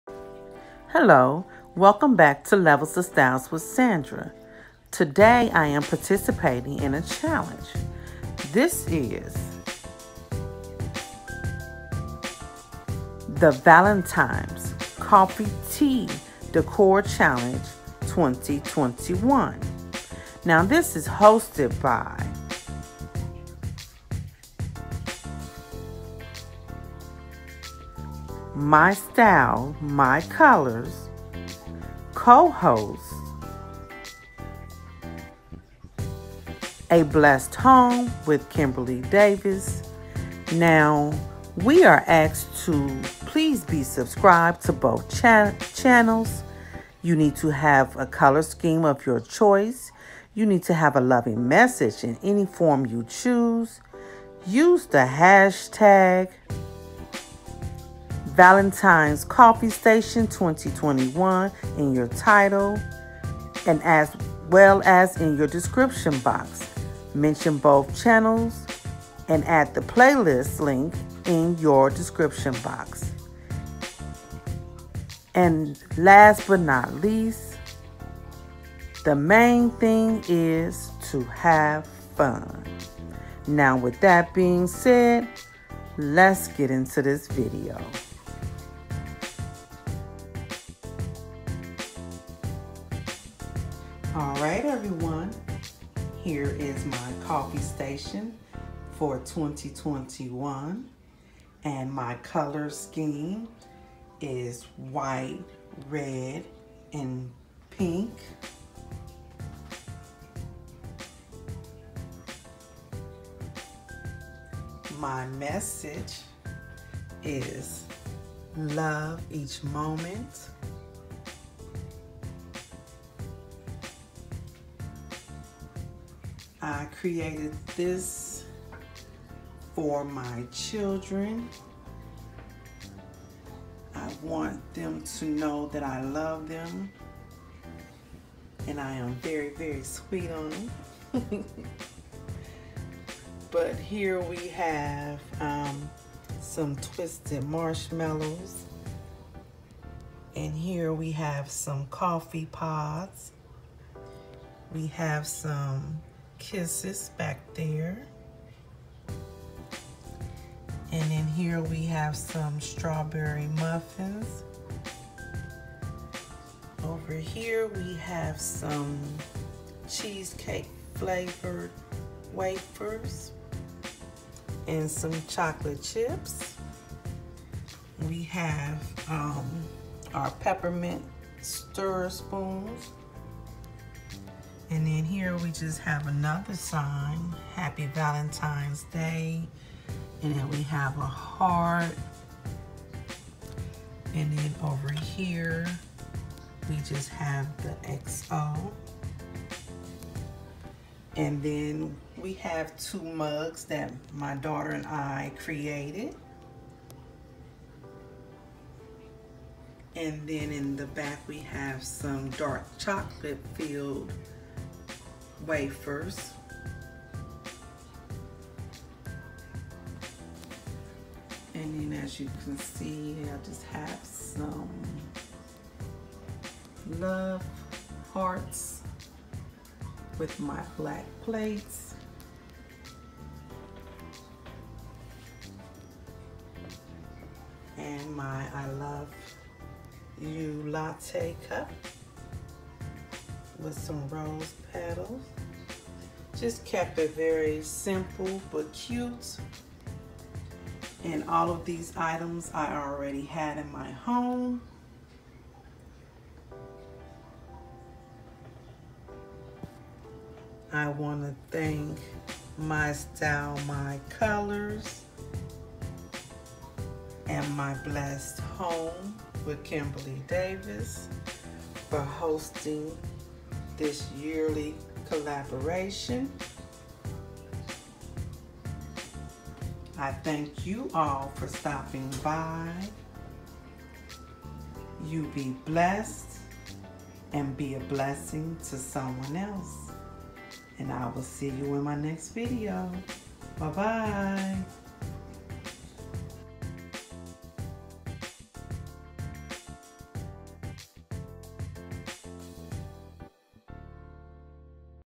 The Valentines Coffee Tea Decor Challenge 2021
Today I am participating in The Valentines Coffee Tea Decor Challenge 2021. This is hosted by MystyleMycolours. Cohost A Blessed Home with Kimberly Davis.

Rules:
2. you must have a color scheme of your choice.
3. you must have a loving message in any form you choose.
4. Please include the hashtags valentinescoffeestation2021 in your title, description box and mention both channels in your video as well as the channels & playlist link in your description box.
5. Most of all HAVE FUN!!!

 Hit The Notification 🛎 So that you'll know when I upload!!

Receive 5$ off 40$ Order
Sandra5